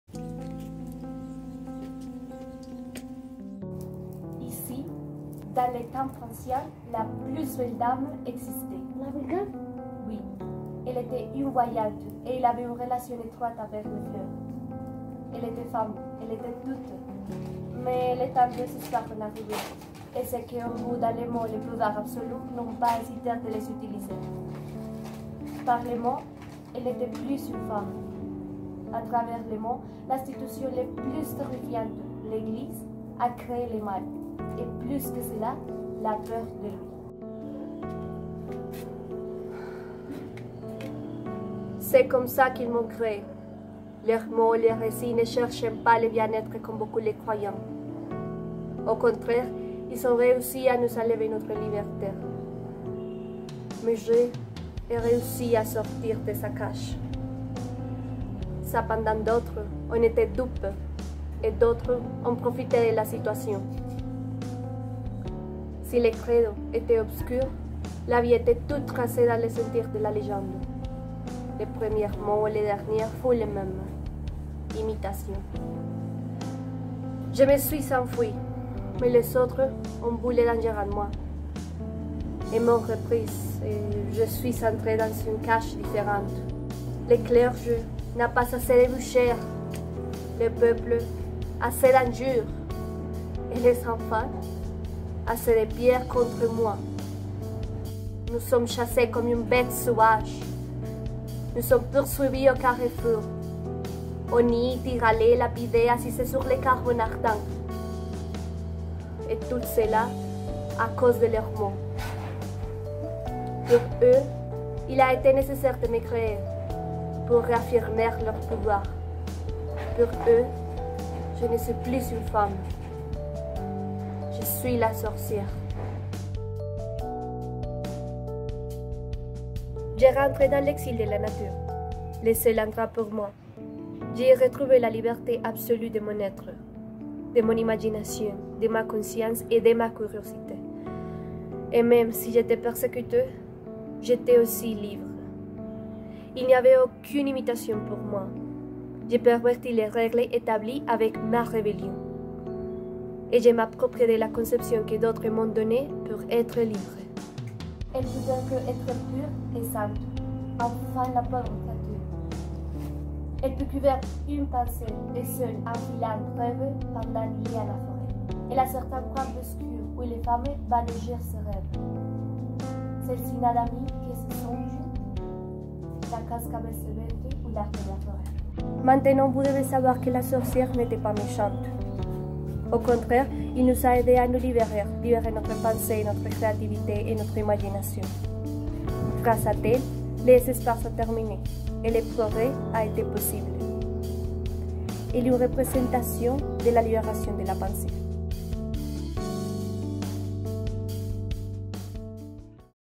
Sketch - Interprétation et Représentation de l'oeuvre La Sorcière chez Jules Michelet
Littérature Franconphone 1
Vidéo finale.
La vidéo contient des extraits pris de l'œuvre